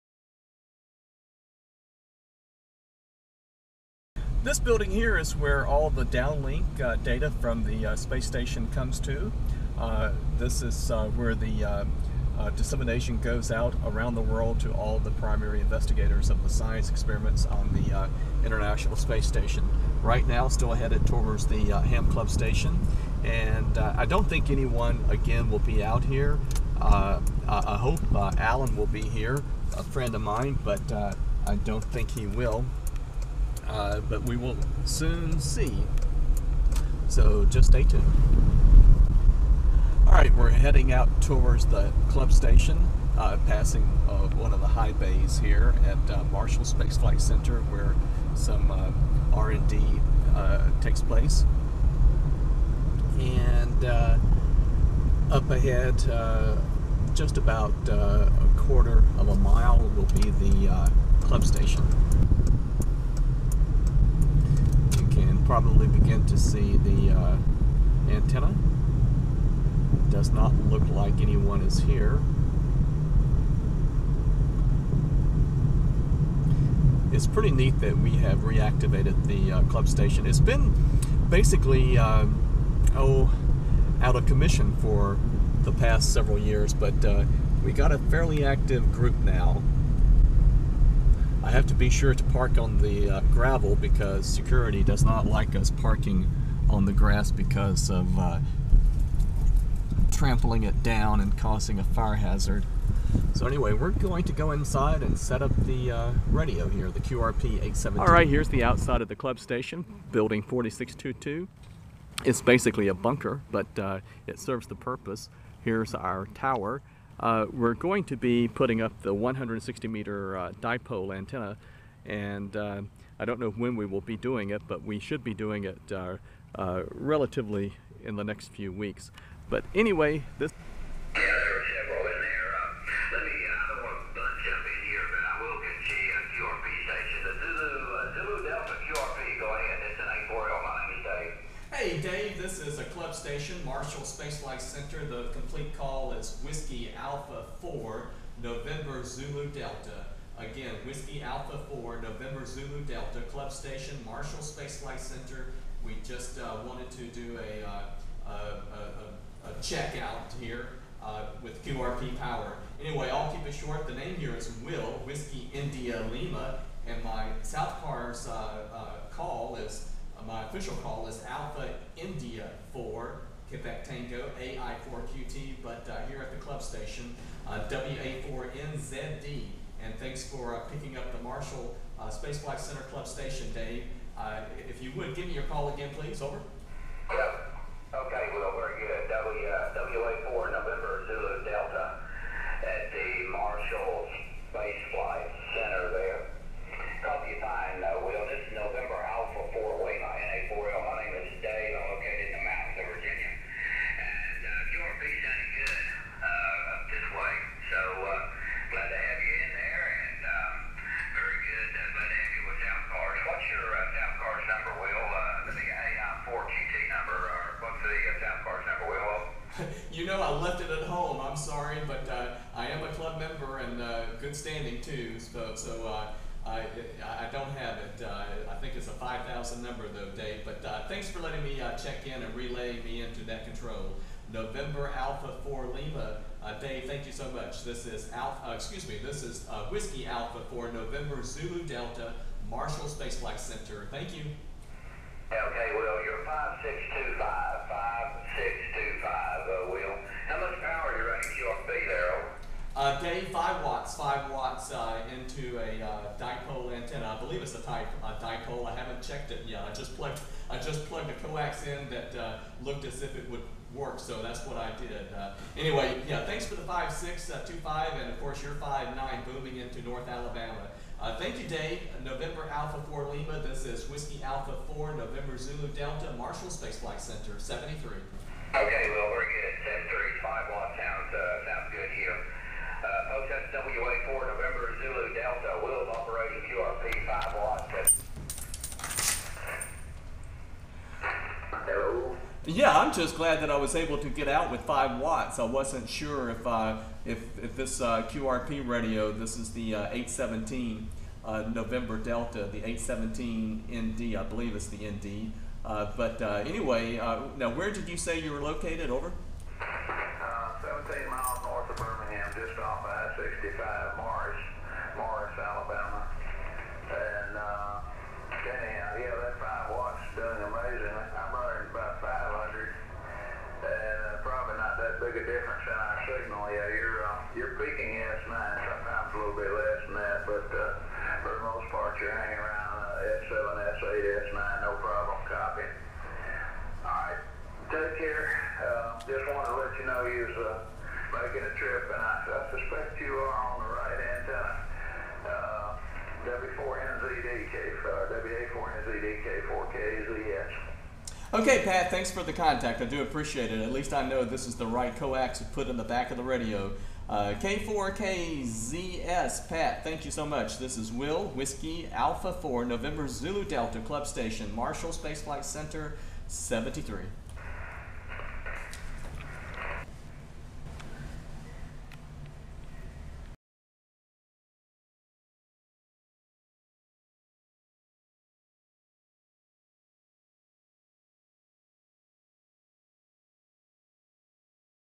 Operating QRP at Club Station WA4NZD
Today I wanted to take my Yaesu 817ND to the Marshall Space Flight Center radio club station to make a few contacts.  Very pleased with the ones I made.  Never underestimate QRP Life is too short NOT to try QRP.